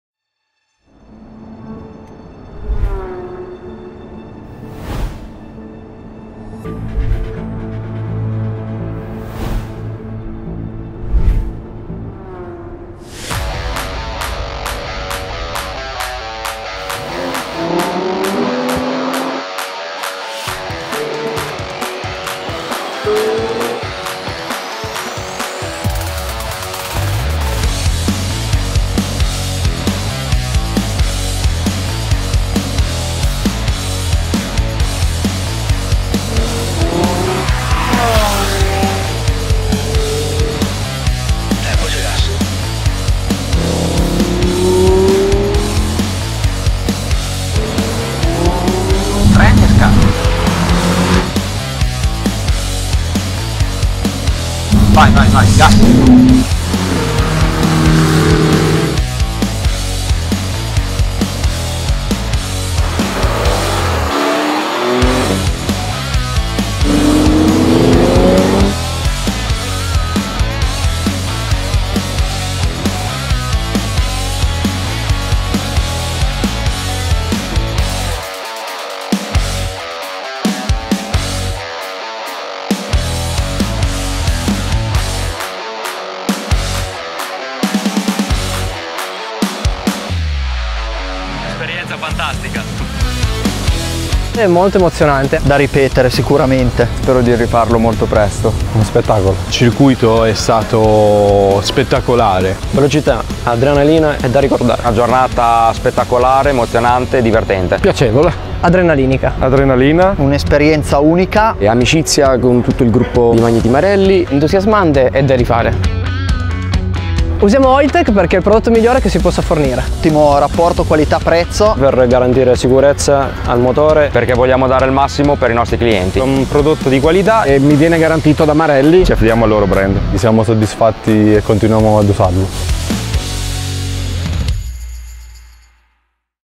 Oiltek Trophy | Imola 2023
Oltre 400 officine si sono date battaglia, partecipando a 20 gare di go-kart organizzate in tutta Italia. Il premio per i 20 vincitori? Una giornata indimenticabile nell’autodromo Enzo e Dino Ferrari di Imola a bordo di Ferrari 458, Ferrari 488, Lamborghini Huracán, Porsche 911 GT3 e Mercedes AMG GT-R per vivere un’esperienza di guida adrenalinica.
Per catturare l'atmosfera unica di queste giornate, non perderti il nostro video ufficiale, in cui i partecipanti raccontano le loro esperienze, offrendoti uno sguardo esclusivo dietro le quinte.

*** EN ***
Over 400 workshops clashed during the 20 go-kart races organized throughout Italy. The prize for the 20 winners? An unforgettable day at the Enzo and Dino Ferrari racetrack in Imola aboard Ferrari 458, Ferrari 488, Lamborghini Huracán, Porsche 911 GT3 and Mercedes AMG GT-R for an adrenaline-pumping driving experience.

To capture the unique atmosphere of these days, don't miss out our official video, in which the participants talk about their experiences, offering you an exclusive look behind the scenes.